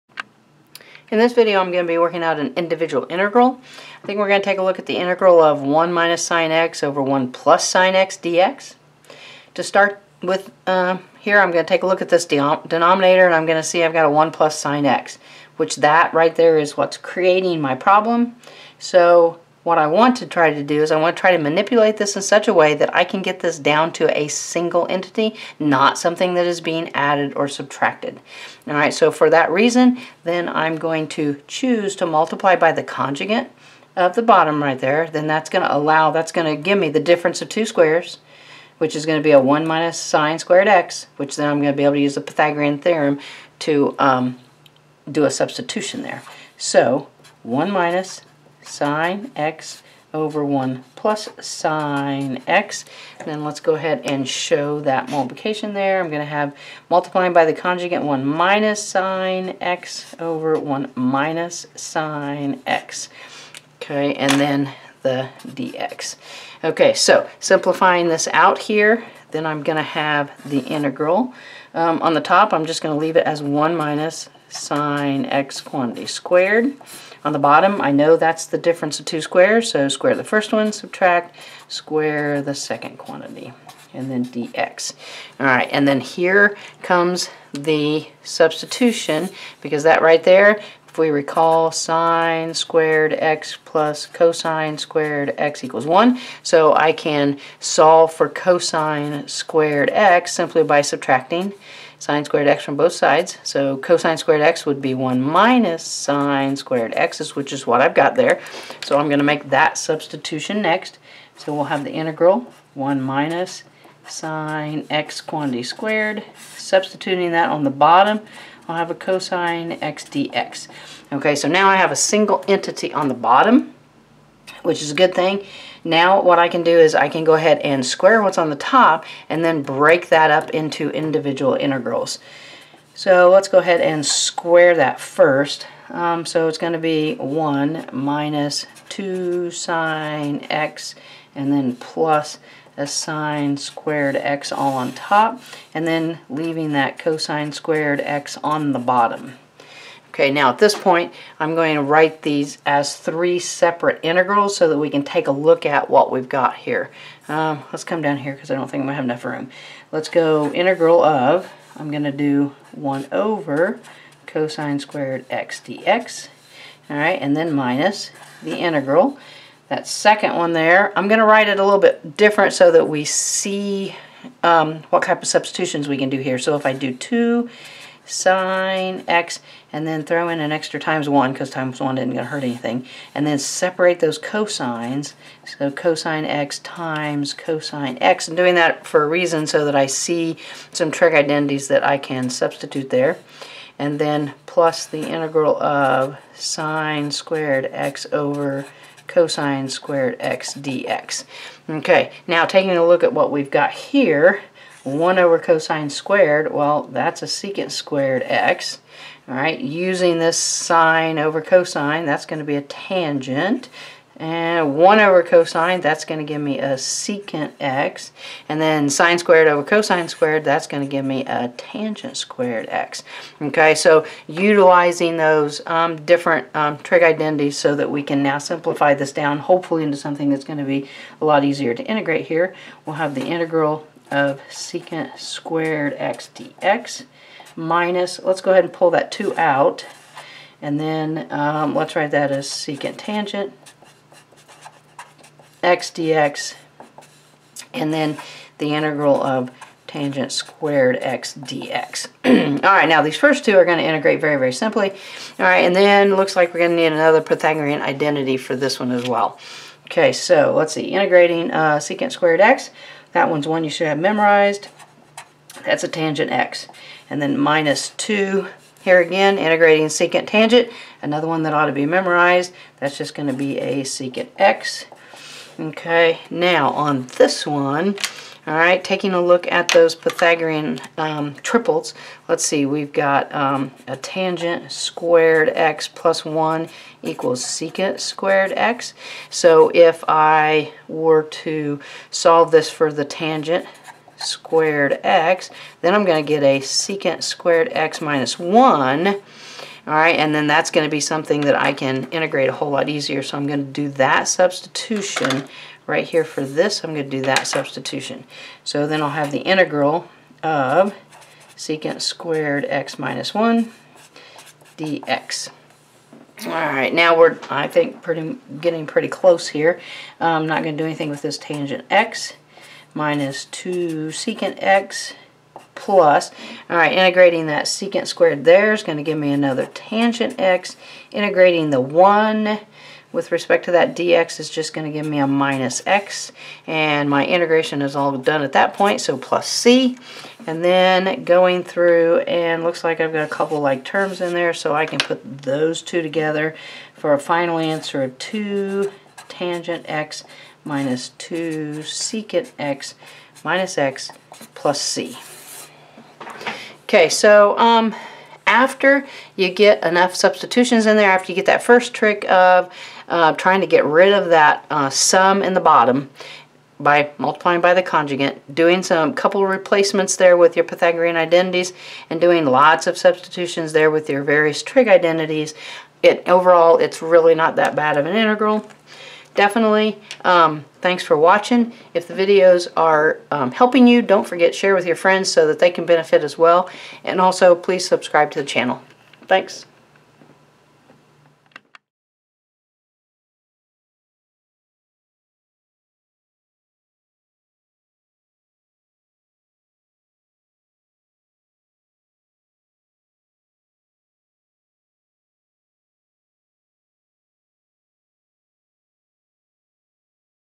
Integral of (1 - sinx)/(1+ sinx) ❖ Calculus 1
This video works out the integral of (1 - sinx)/(1+ sinx).  This type of integral is typically found in a Calculus 1 class.